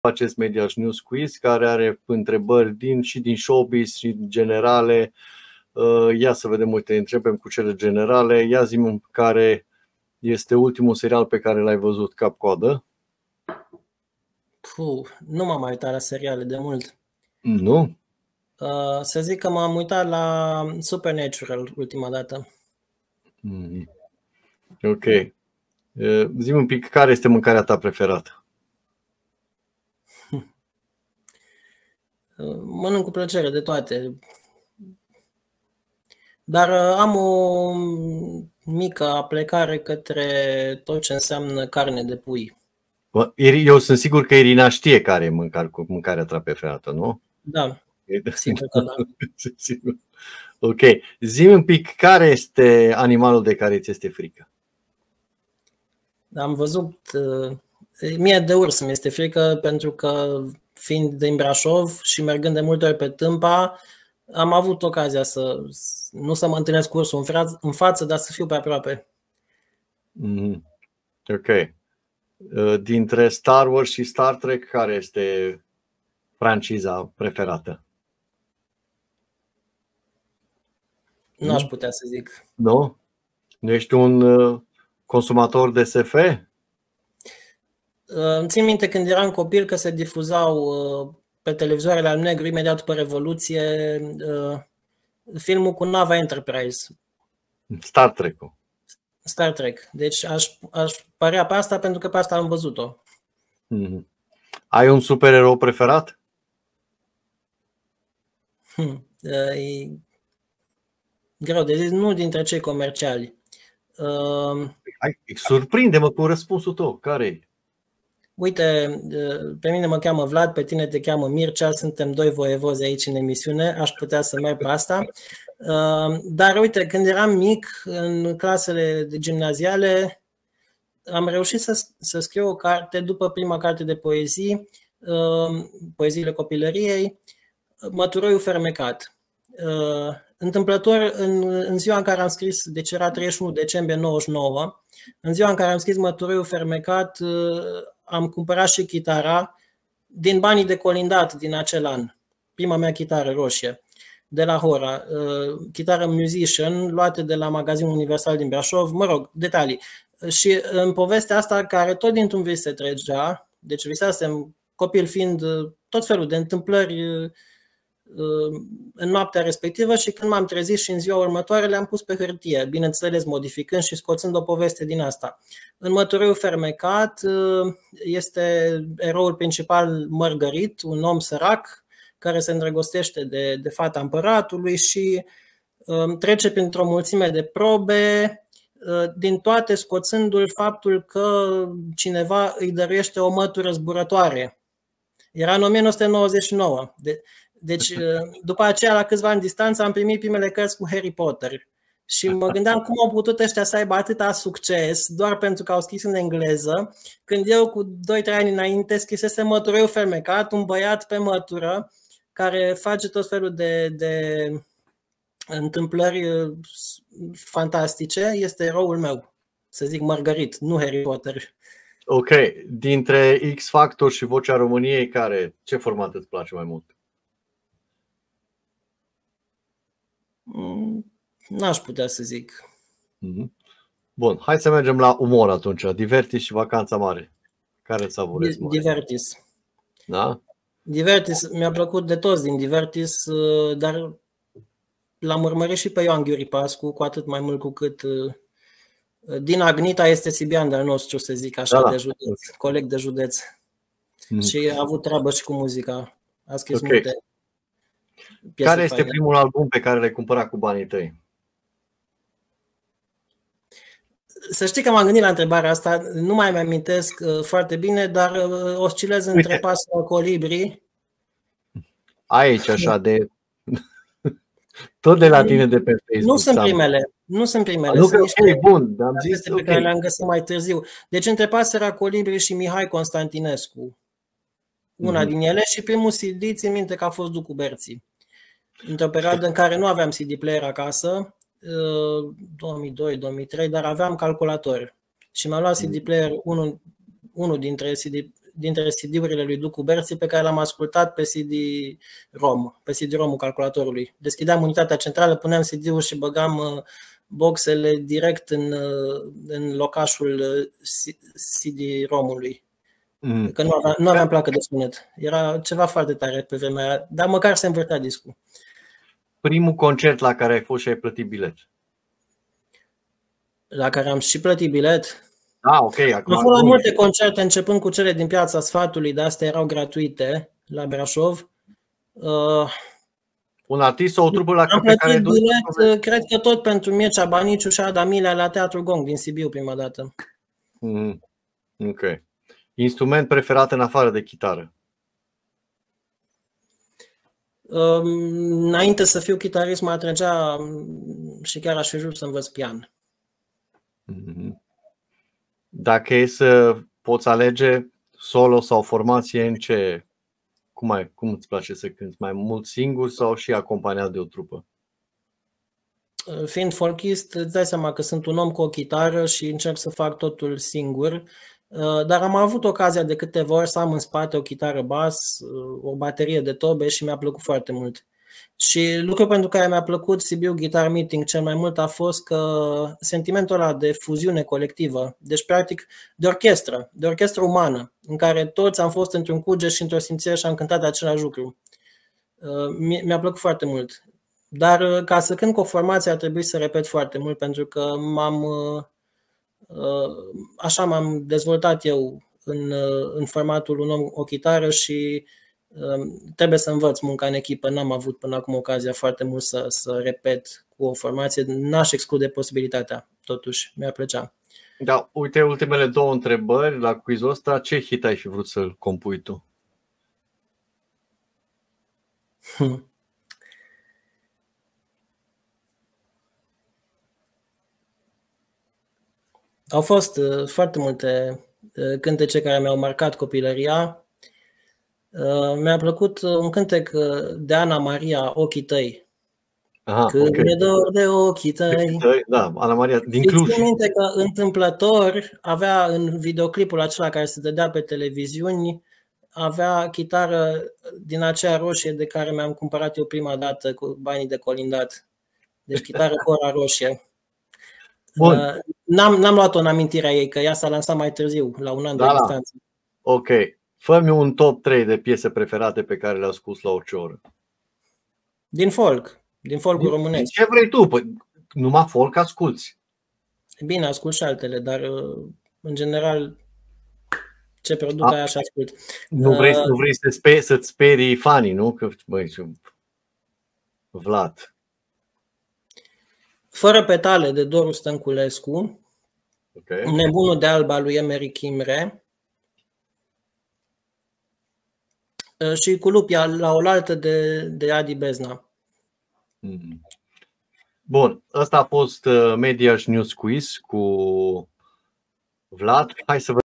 Medias News Quiz cu Vlad Pârău
Vlad Pârău a răspuns întrebărilor de la Mediaș News Quiz.